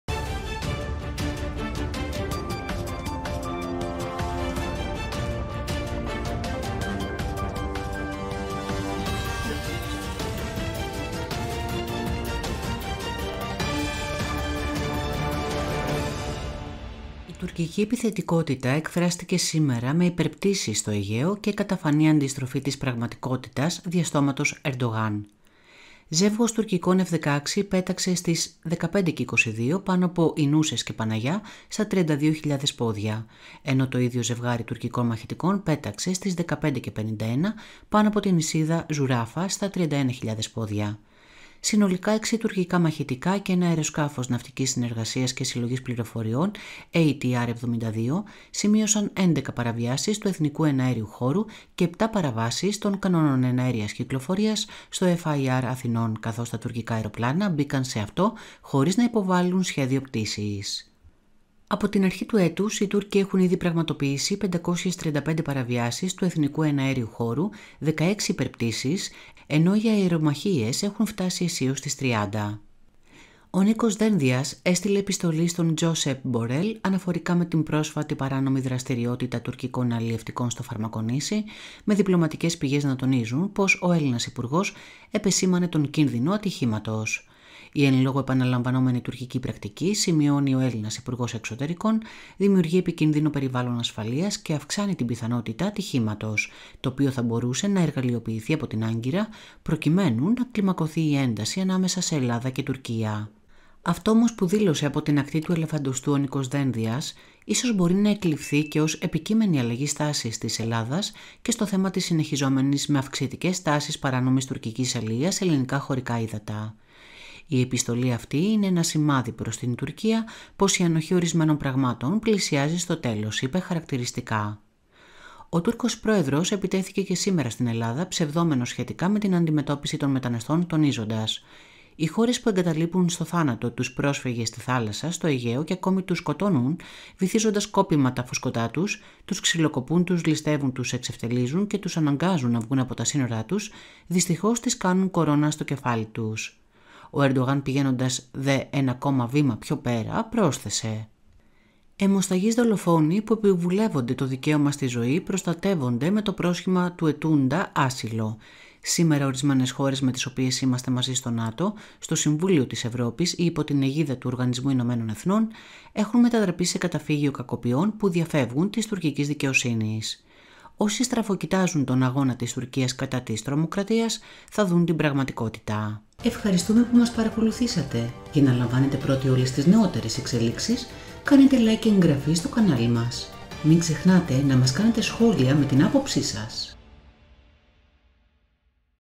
«Κεραυνοί» Δένδια από Ακτή του Ελεφαντοστού: Η ανοχή προς την Τουρκία πλησιάζει στο τέλος της
Από την αρχή του έτους οι Τούρκοι έχουν ήδη πραγματοποιήσει 535 παραβιάσεις του Εθνικού Εναέριου Χώρου, 16 υπερπτήσεις, ενώ οι αερομαχίες έχουν φτάσει αισίως τις 30…

Ο Νίκος Δένδιας έστειλε επιστολή, στον, Josep Borrell, αναφορικά με την πρόσφατη παράνομη δραστηριότητα τουρκικών αλιευτικών στο Φαρμακονήσι με διπλωματικές πηγές να τονίζουν πως ο Έλληνας υπουργός επεσήμανε τον κίνδυνο «ατυχήματος»:

«H εν λόγω επαναλαμβανόμενη τουρκική πρακτική, σημειώνει ο Έλληνας Υπουργός Εξωτερικών, δημιουργεί επικίνδυνο περιβάλλον ασφαλείας και αυξάνει την πιθανότητα «ατυχήματος», το οποίο θα μπορούσε να εργαλειοποιηθεί από την Άγκυρα προκειμένου να κλιμακωθεί η ένταση ανάμεσα σε Ελλάδα και Τουρκία».

Κεφάλαια:
0:00 - Εισαγωγή
0:18 - «Κεραυνός» Δένδια από την Αφρική

Η Τουρκική επιθετικότητας εκφράστηκε σήμερα με υπερπτήσεις στο Αιγαίο και καταφανή αντιστροφή της πραγματικότητας δια στόματος Ερντογάν.

Ζεύγος τουρκικών F-16 πέταξε στις 15:22 πάνω από Οινούσσες και Παναγιά στα 32.000 πόδια ενώ το ίδιο ζευγάρι τουρκικών μαχητικών πέταξε στις 15:51 πάνω από την νησίδα Ζουράφα στα 31.000 πόδια. Συνολικά 6 τουρκικά μαχητικά και ένα αεροσκάφος ναυτικής συνεργασίας και συλλογής πληροφοριών ATR-72 σημείωσαν 11 παραβιάσεις του Εθνικού Εναέριου Χώρου και 7 παραβάσεις των κανόνων εναέριας κυκλοφορίας στο FIR Αθηνών καθώς τα τουρκικά αεροπλάνα μπήκαν σε αυτό χωρίς να υποβάλλουν σχέδιο πτήσης.

Ο Νίκος Δένδιας έστειλε επιστολή, στον, Josep Borrell, αναφορικά με την πρόσφατη παράνομη δραστηριότητα τουρκικών αλιευτικών στο Φαρμακονήσι με διπλωματικές πηγές να τονίζουν πως ο έλληνας υπουργός επεσήμανε τον κίνδυνο «ατυχήματος»: «H εν λόγω επαναλαμβανόμενη τουρκική πρακτική, σημειώνει ο Έλληνας Υπουργός Εξωτερικών, δημιουργεί επικίνδυνο περιβάλλον ασφαλείας και αυξάνει την πιθανότητα «ατυχήματος», το οποίο θα μπορούσε να εργαλειοποιηθεί από την Άγκυρα προκειμένου να κλιμακωθεί η ένταση ανάμεσα σε Ελλάδα και Τουρκία».

Αυτό όμως που δήλωσε από την Ακτή του Ελεφαντοστού ο Νίκος Δένδιας ίσως μπορεί να εκληφθεί και ως επικείμενη αλλαγή στάσης της Ελλάδας και στο θέμα της συνεχιζόμενης με αυξητικές τάσεις παράνομης τουρκικής αλιείας σε ελληνικά χωρικά ύδατα

«Η επιστολή αυτή είναι ένα σημάδι προς την Τουρκία πως η ανοχή ορισμένων πραγμάτων πλησιάζει στο τέλος» είπε χαρακτηριστικά.

Ο τούρκος πρόεδρος επιτέθηκε και σήμερα στην Ελλάδα ψευδόμενος σχετικά με την αντιμετώπιση των μεταναστών τονίζοντας: «Οι χώρες που εγκαταλείπουν στο θάνατο, τους πρόσφυγες στη θάλασσα, στο Αιγαίο, κι ακόμη τους σκοτώνουν βυθίζοντας σκόπιμα τα φουσκωτά τους, τους ξυλοκοπούν, τους ληστεύουν, τους εξευτελίζουν και τους αναγκάζουν να βγουν από τα σύνορά τους, δυστυχώς τις κάνουν κορώνα στο κεφάλι τους».

Ο Ερντογάν πηγαίνοντας δε ένα ακόμα βήμα πιο πέρα πρόσθεσε: «Αιμοσταγείς δολοφόνοι που επιβουλεύονται το δικαίωμα στη ζωή προστατεύονται με το πρόσχημα του αιτούντα άσυλο. Σήμερα, ορισμένες χώρες με τις οποίες είμαστε μαζί στο ΝΑΤΟ, στο Συμβούλιο της Ευρώπης ή υπό την αιγίδα του ΟΗΕ, έχουν μετατραπεί σε καταφύγιο κακοποιών που διαφεύγουν της τουρκικής δικαιοσύνης. Όσοι στραβοκοιτάζουν τον αγώνα της Τουρκίας κατά της τρομοκρατίας, θα δουν την πραγματικότητα.»